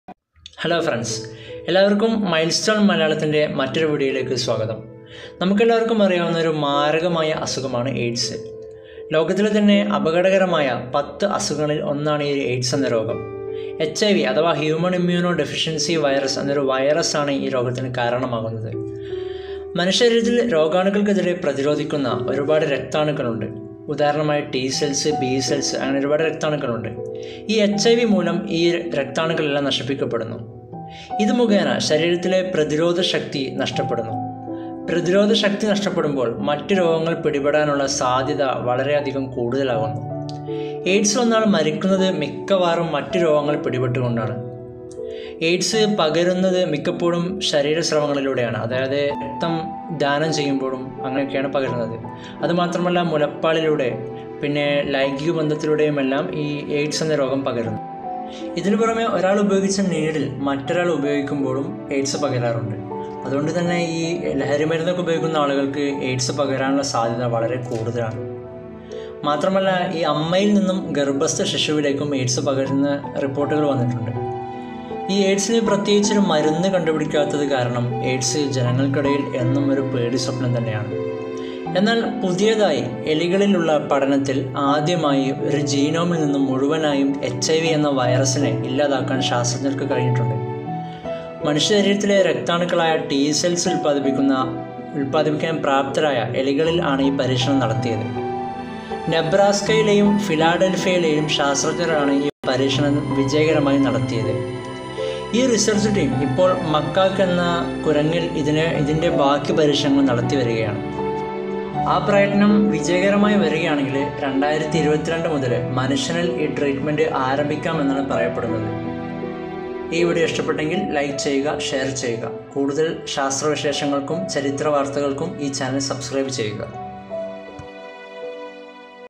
Scientists successfully invented medicine for AIDS, Milestone Malayalam, science news, virus
എയ്ഡ്സ് രോഗത്തിനുള്ള മരുന്ന് നെബ്രാസ്ക യിലേയും ഫിലാഡൽഫിയ യിലേയും ശാസ്ത്രജഞർ കണ്ടുപിടിക്കുന്നു..